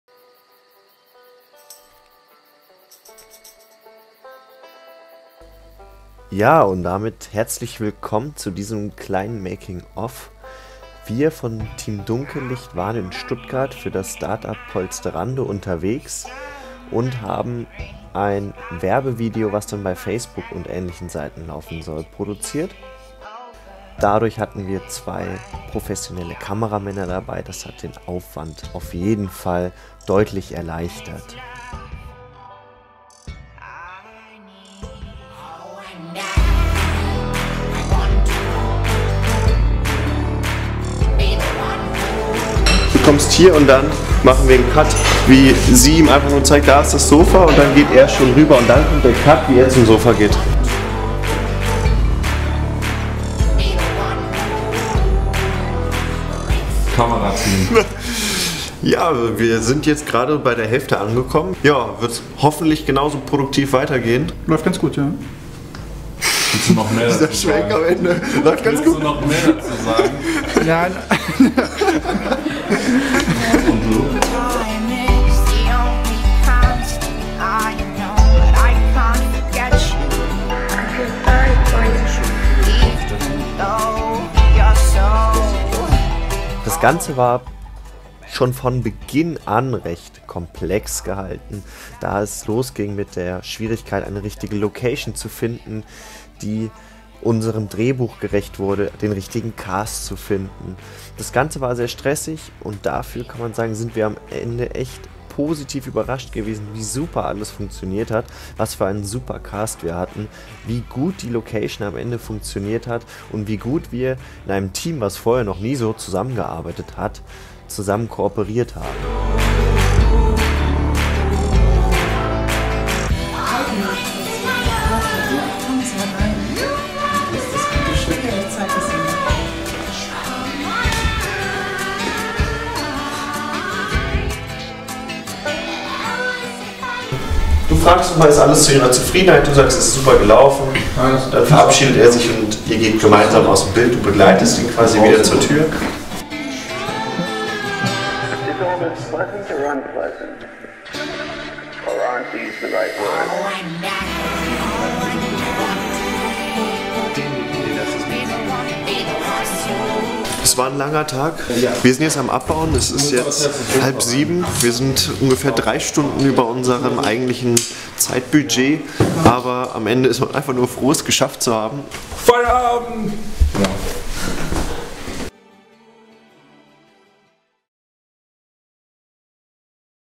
Making Of Polsterando - 2018
Making Of zum ersten großen Projekt im Jahr 2018.
Danke an das Team und den Cast!

Non-Commercial!

– WEITERE INFORMATIONEN –

Vielen Dank für Dein Interesse!
oder auf unserem YouTube-Kanal!

2016 dunkellicht
dunkellicht film